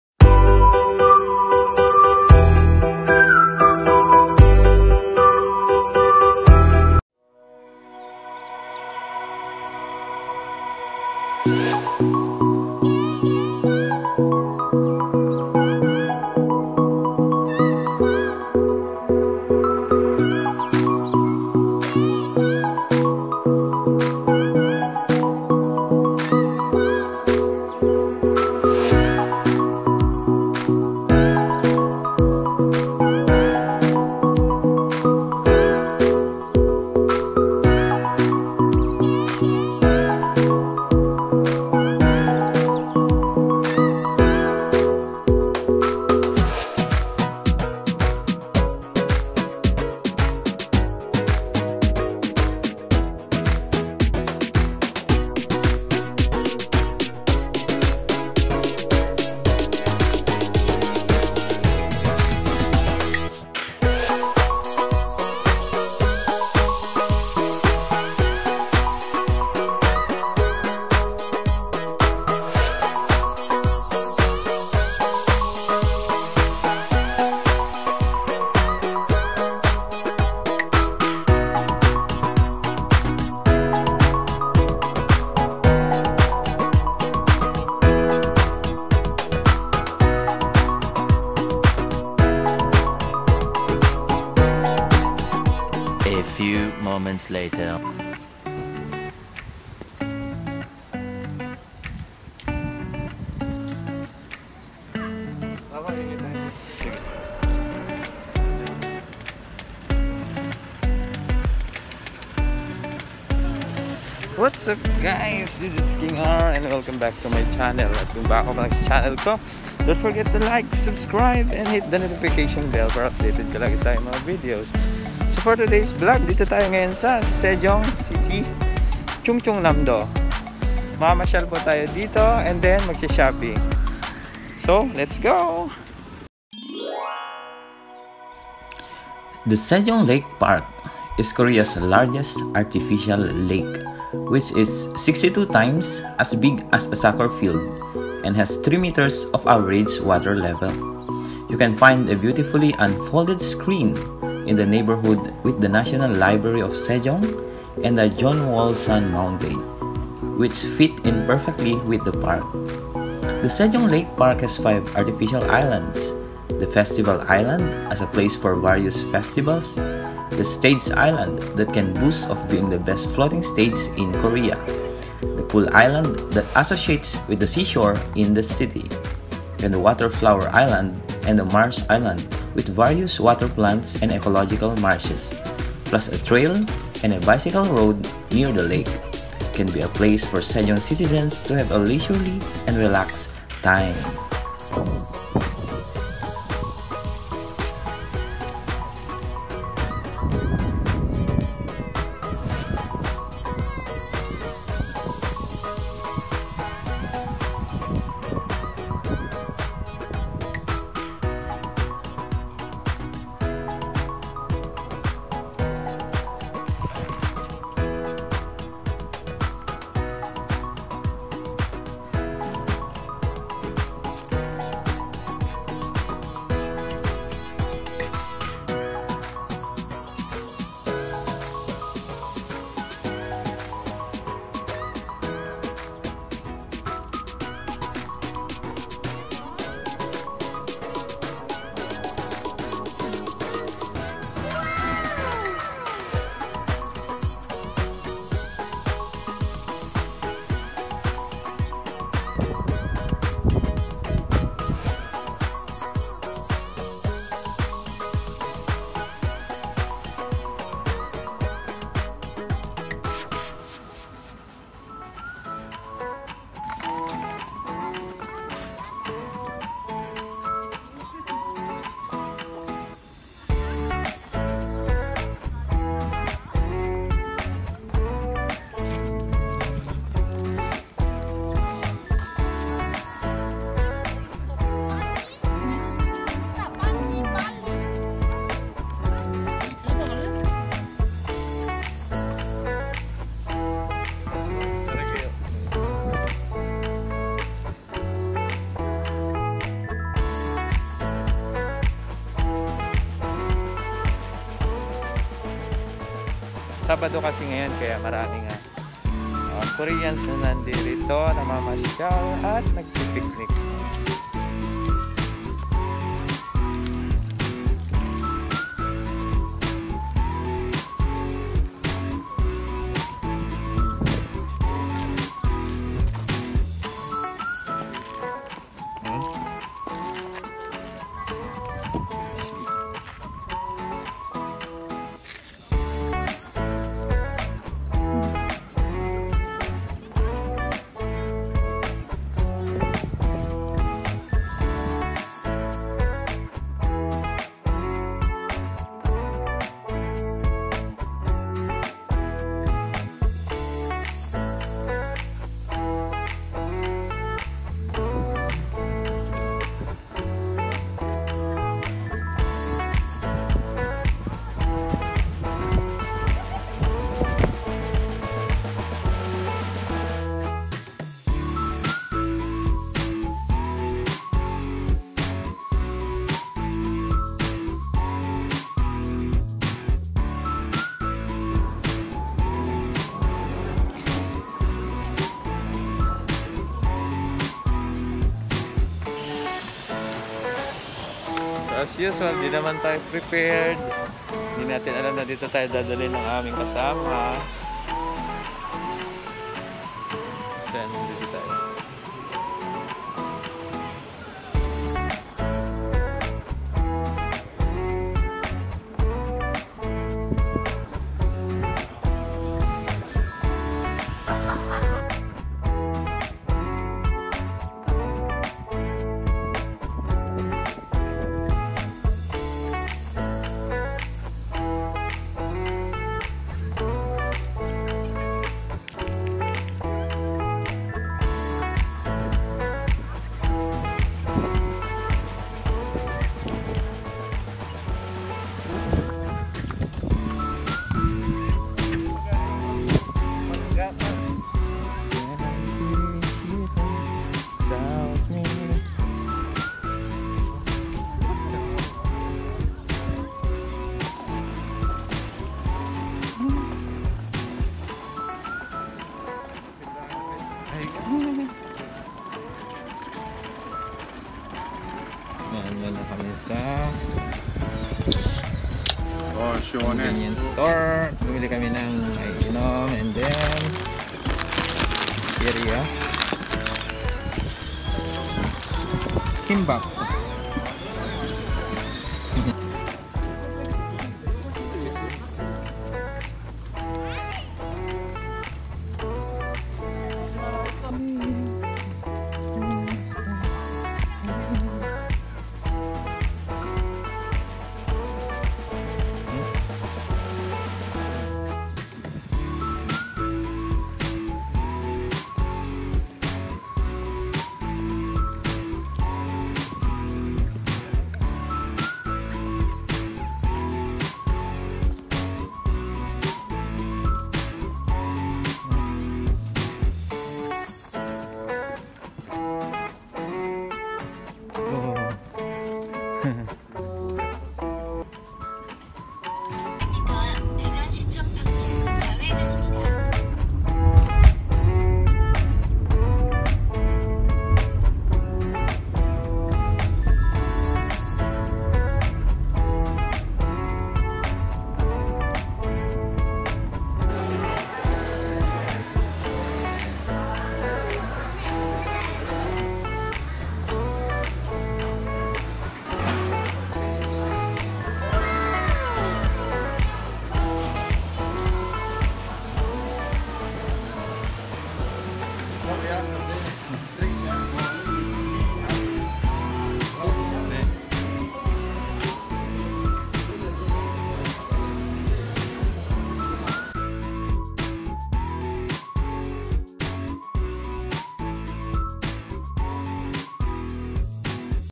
Sejong Lake Park Tour |South Korea l KingR Travel
The Sejong Lake Park has Korea’s LARGEST ARTIFICIAL LAKE, which is 62 times as big as a soccer field and has 3m of average water level. You can find a beautifully unfolded screen in the neighborhood with the National Library of Sejong and the Jeonwolsan Mountain, which fit in perfectly with the park. The Sejong Lake Park has five artificial islands: the Festival Island as a place for various festivals, the Stage Island that can boast of being the best floating stage in Korea, the Pool Island that associates with the seashore in the city, and the Water Flower Island and the Marsh Island with various water plants and ecological marshes. Plus, a trail and bicycle road near the lake can be a place for Sejong citizens to have a leisurely and relaxed time.